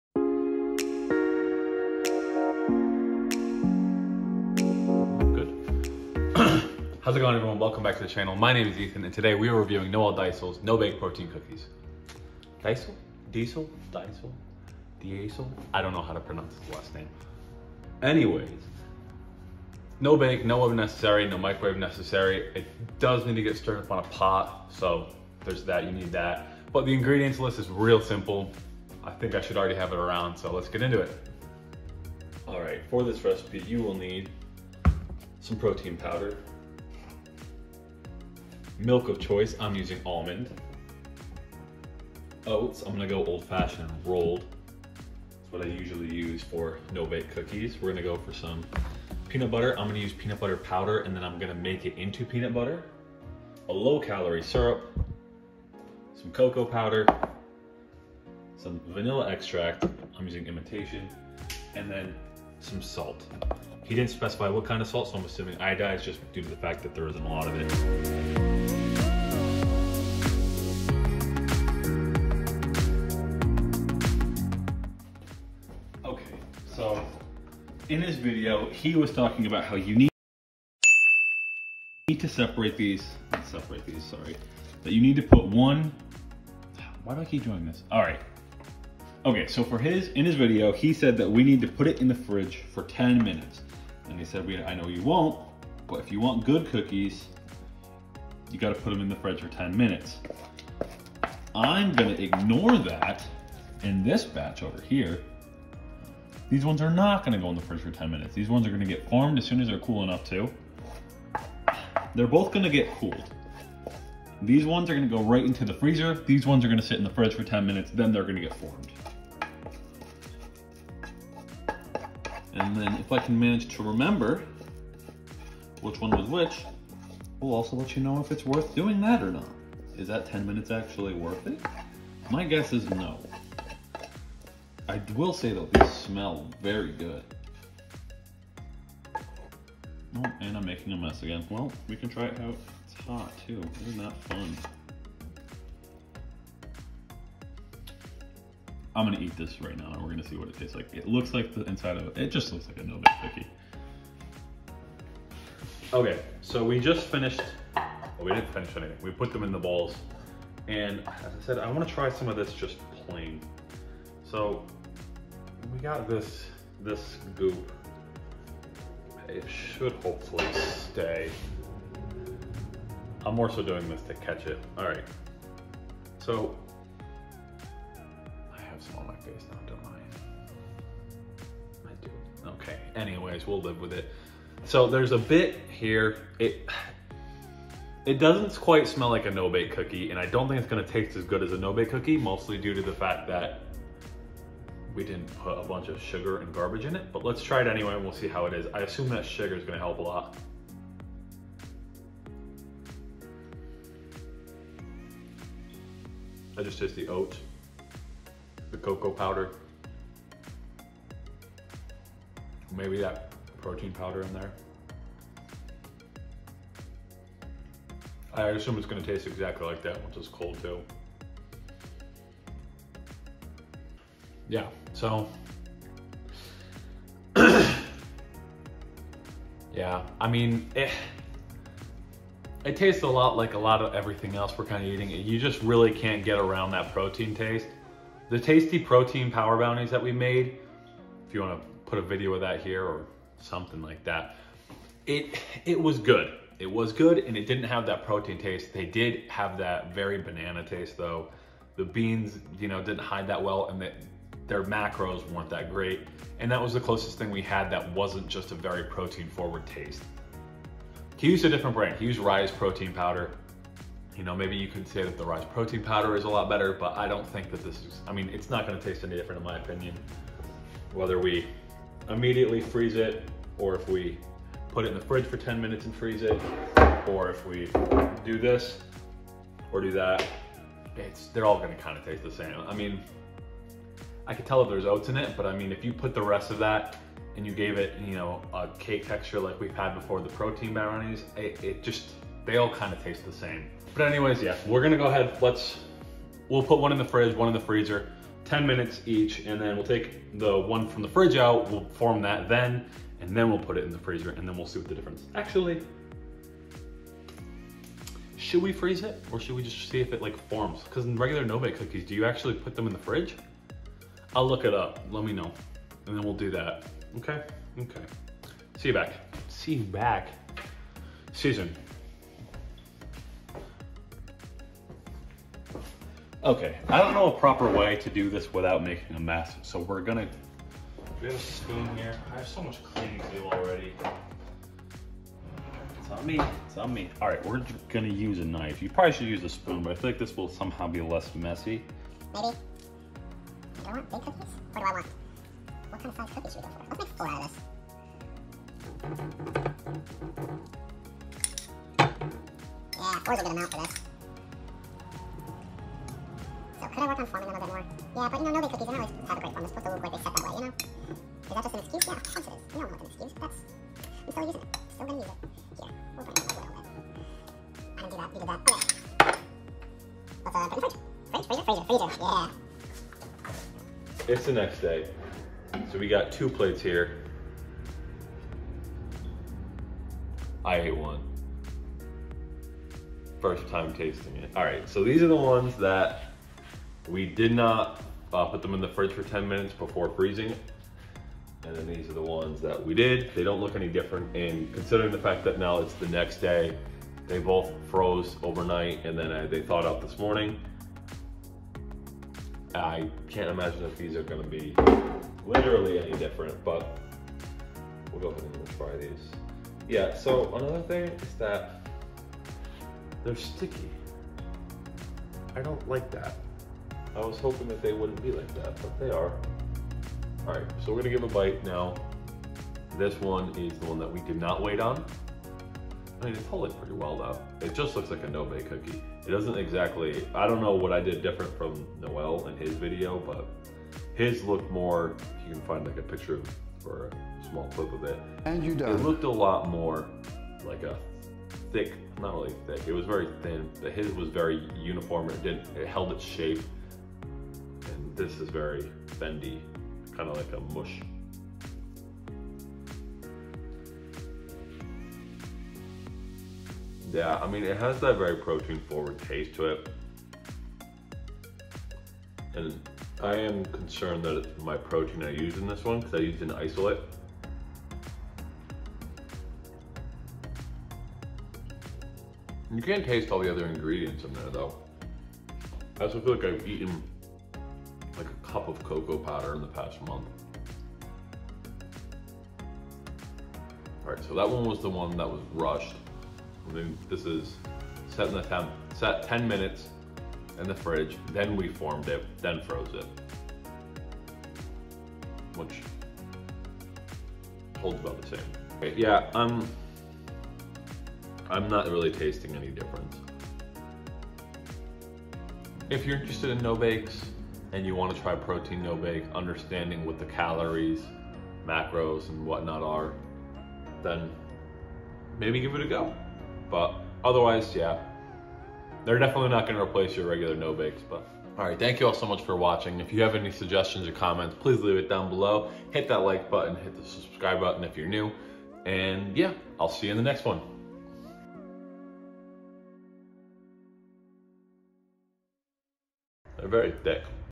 Protein no-bake cookies? | Eats Eats Ep. 4 | NOEL DEYZEL Protein Cookie Review
Support me and get some of my favorite supplements!